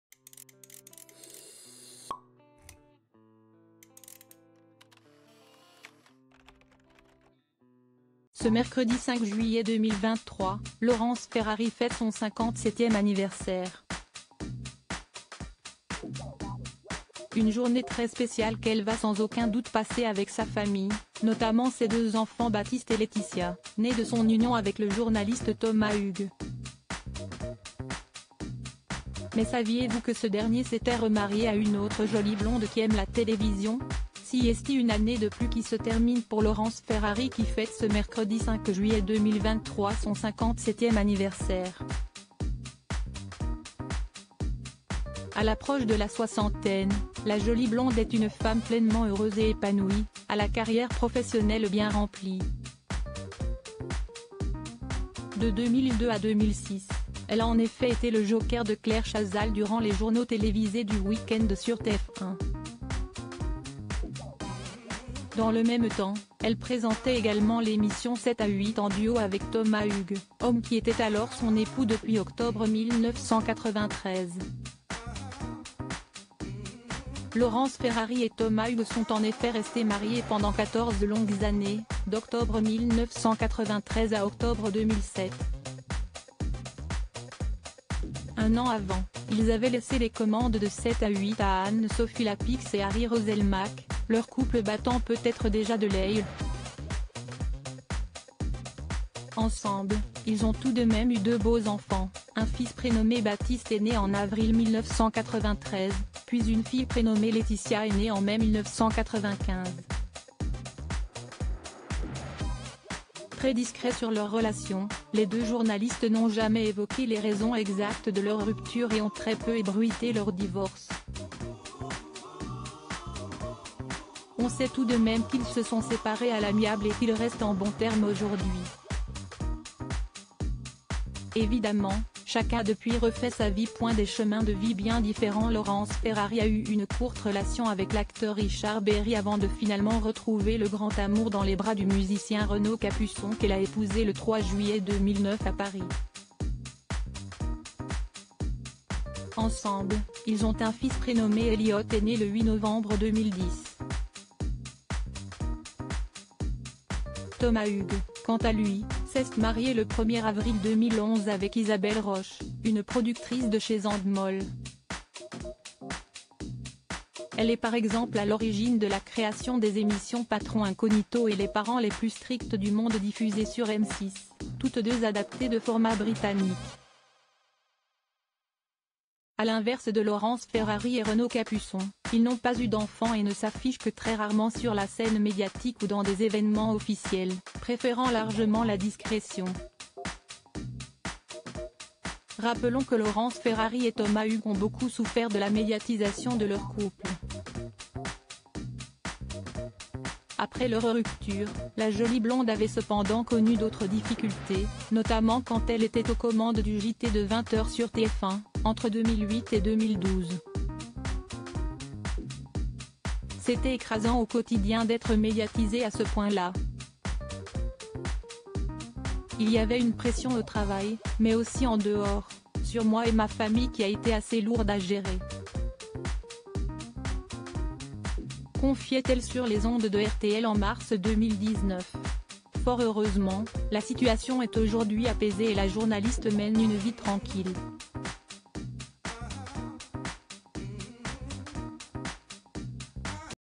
PHOTO Laurence Ferrari divorcée de Thomas Hugues : le journaliste remarié à une autre jolie blonde q
PHOTO Laurence Ferrari divorcée de Thomas Hugues : le journaliste remarié à une autre jolie blonde qui aime la télévision !